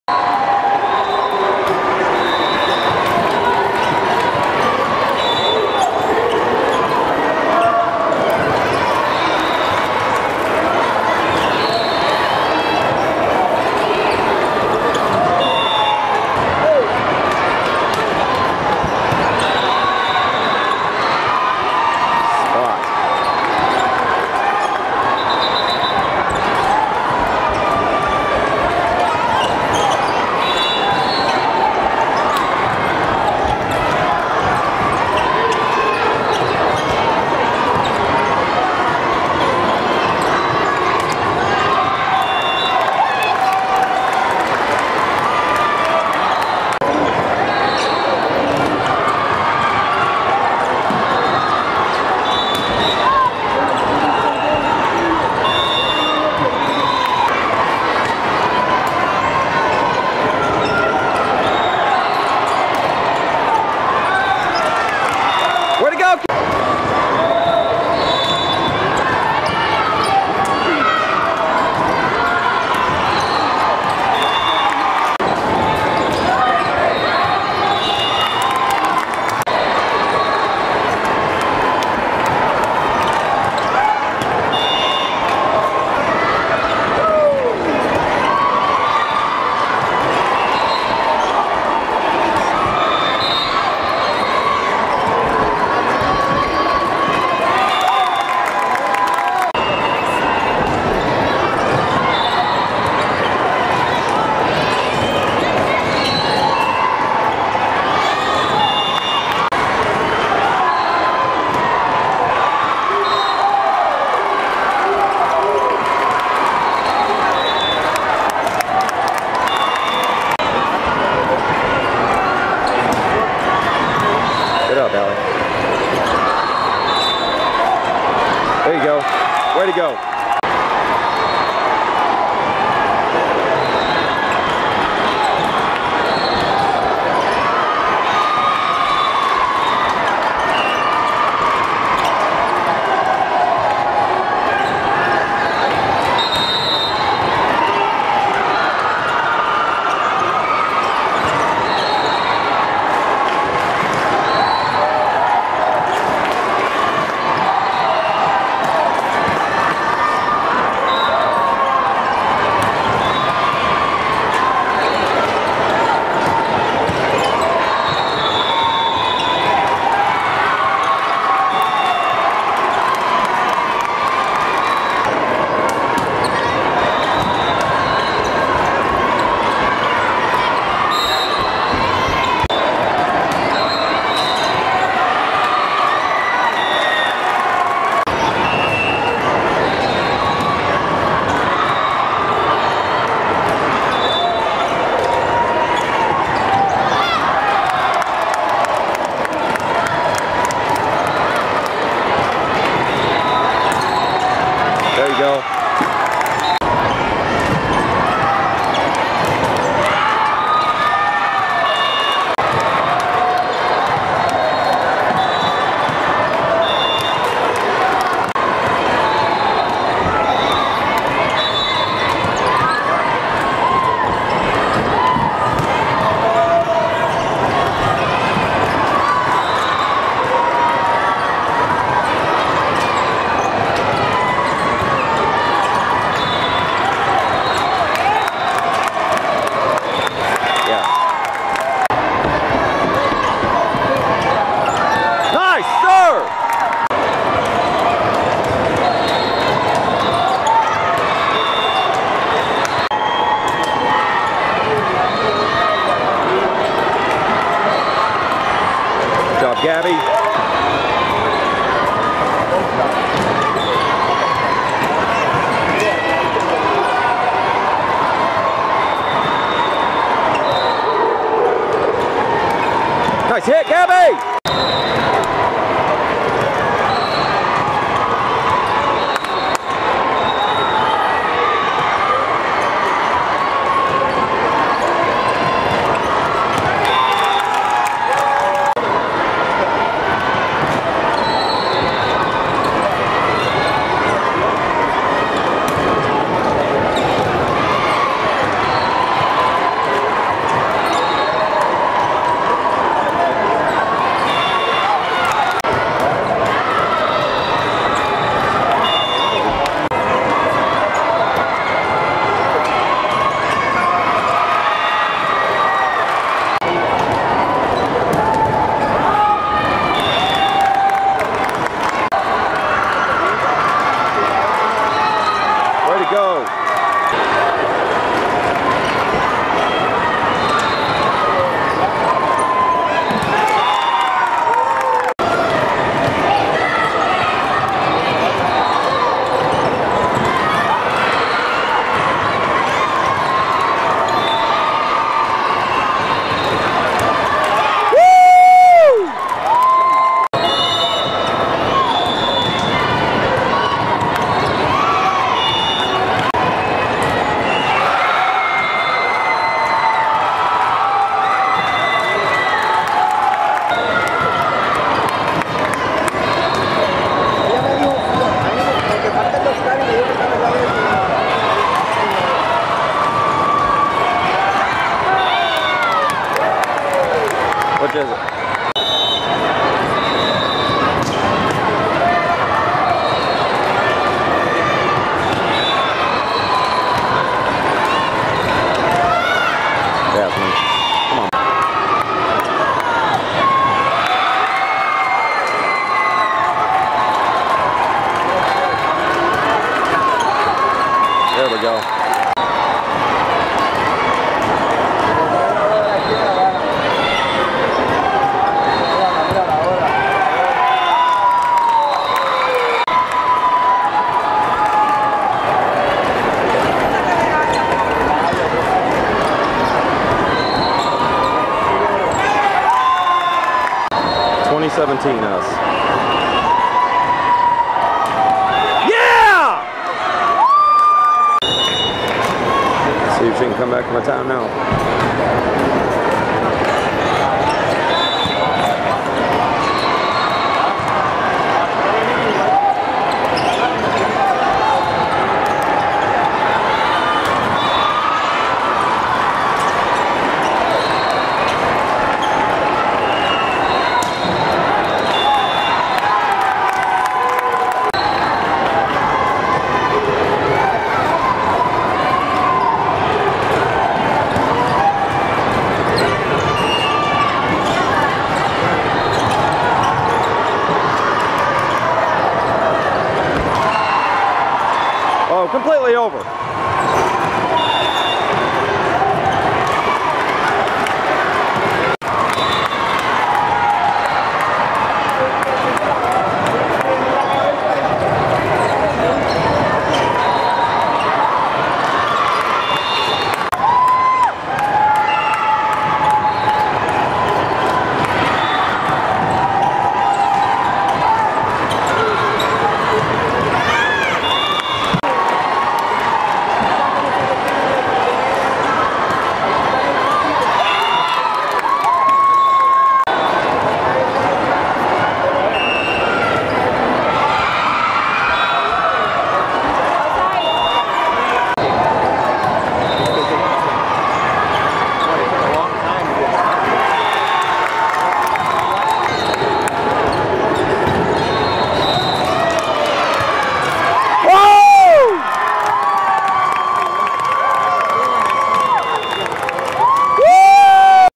AAU Day 4 Match 2 Set 2
AAU Girl's Junior National Championship. June 19, 2010 at the Wide World of Sports.  Match 2, Set 2 v. Corozal Volleyball Academy Malvin